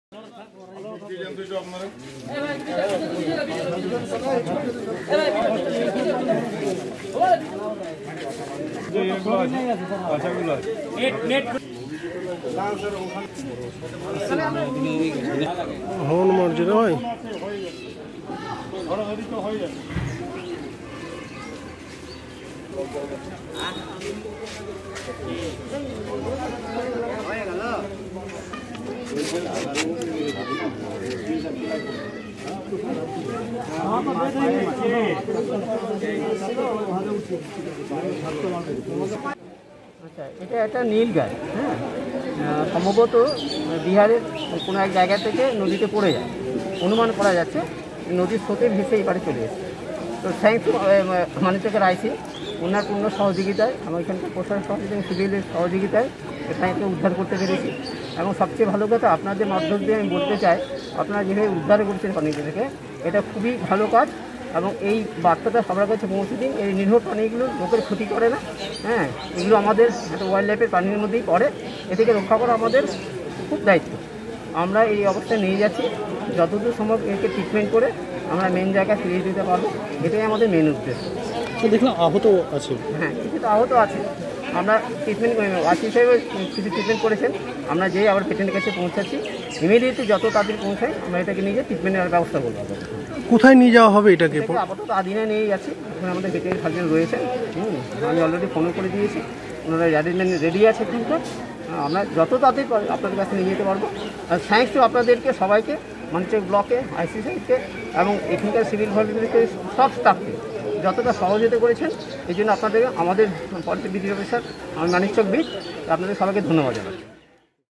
Nilgai rescued in Manikchak
The rescue of a nilgai on the banks of the Ganges at Domhat Haddatola village in Manikchak on monday caused a stir in the region. Police rescued the animal with the help of locals. Later, the nilgai was handed over to the forest department by the police. According to local sources, the injured nilgai was stuck close to the bank of the river. The locals tried to rescue the nilgai as soon as they saw it.